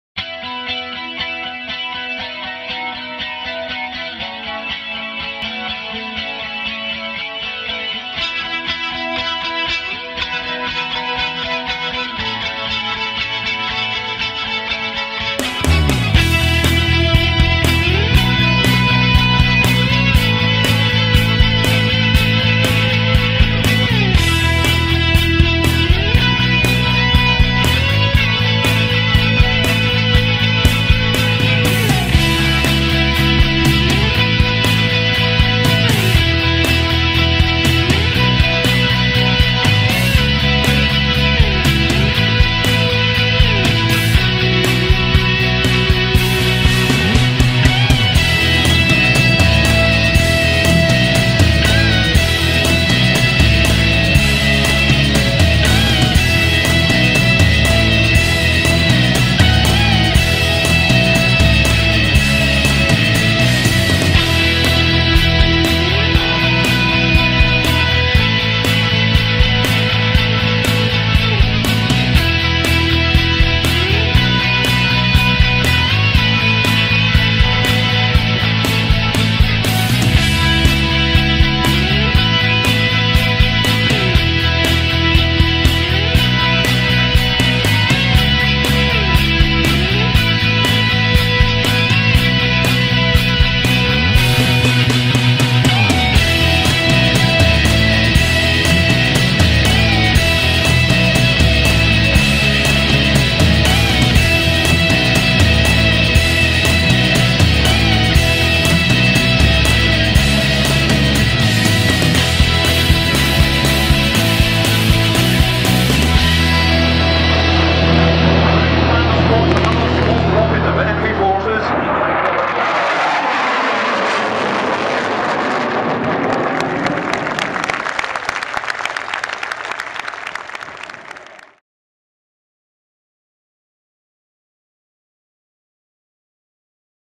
This is Aviation at its BEST!! 1080p HD
All footage obtained by myself at Farnborough International Airshow 2012.